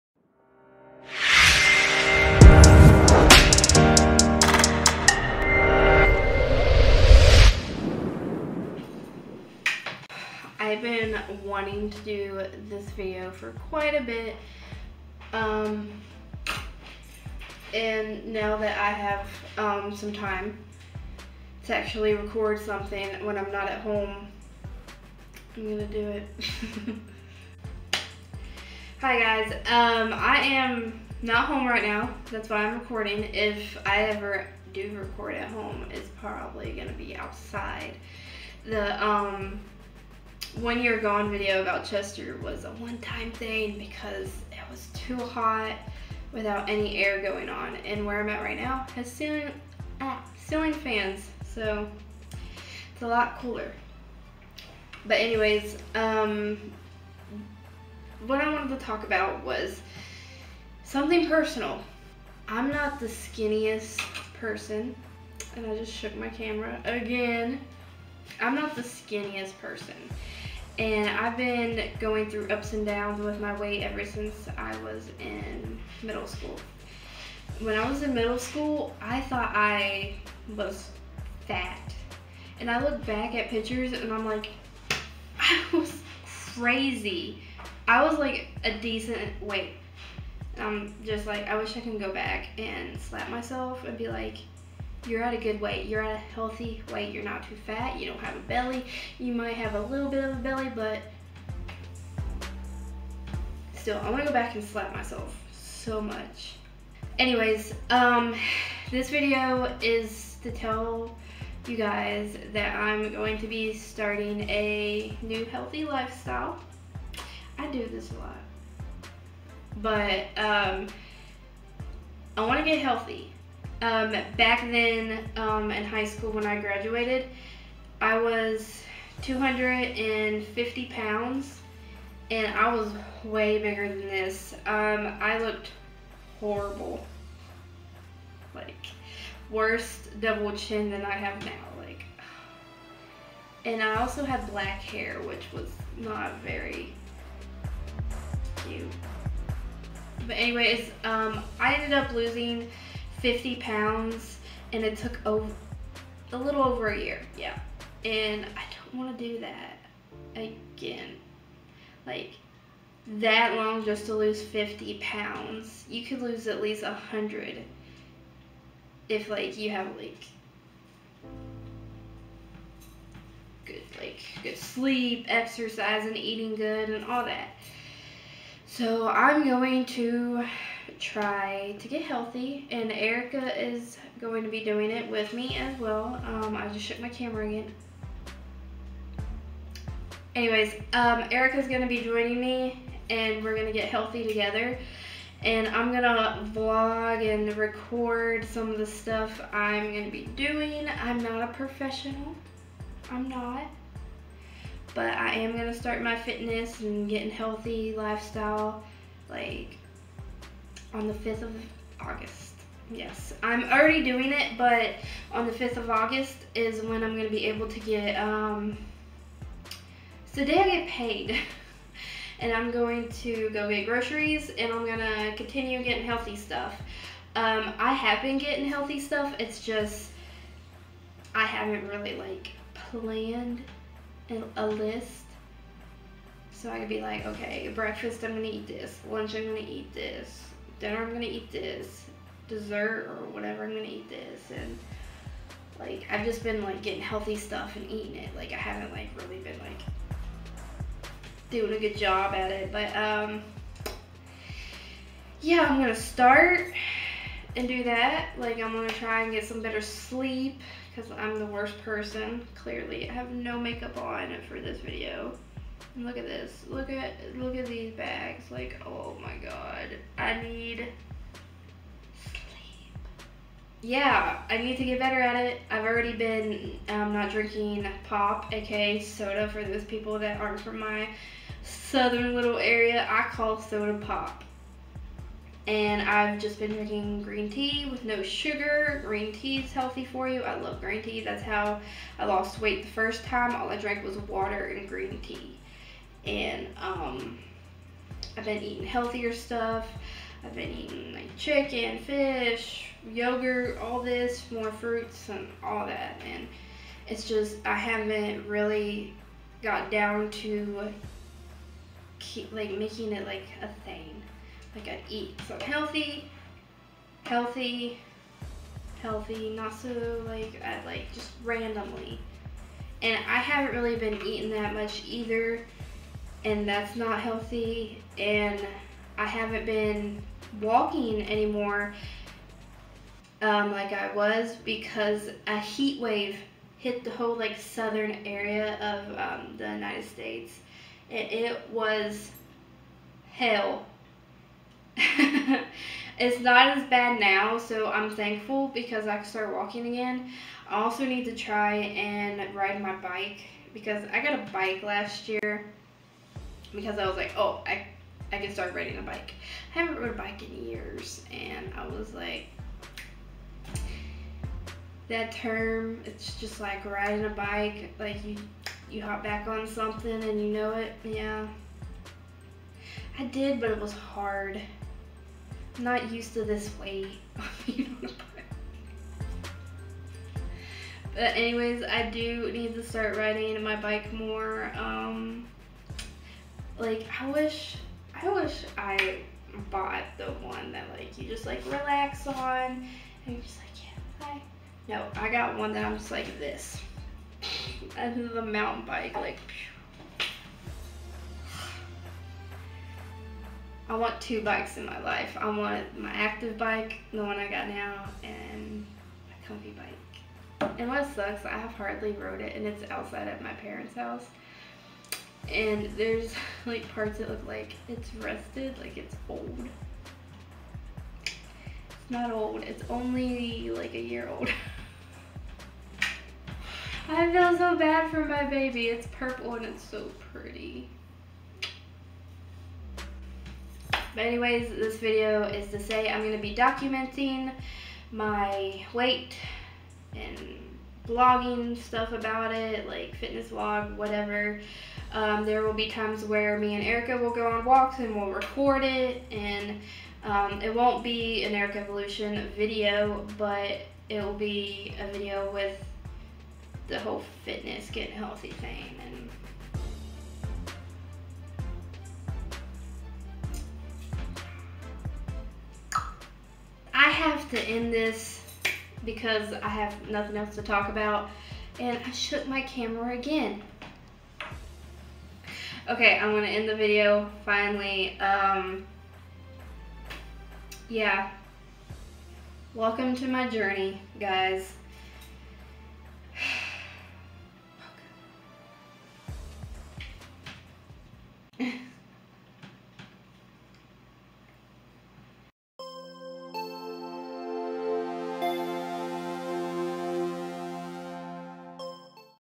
My Healthy Journey BEGINS| LPfreakWWE
I've been wanting to do this video for a bit talking about how I want to start my healthy life style again. I have started my journeys before, but this time I really need to do this for myself. I put this out for everyone to see so I need to stick to it. If anyone has any advise feel free to tell me I don't mind some helpful ideas.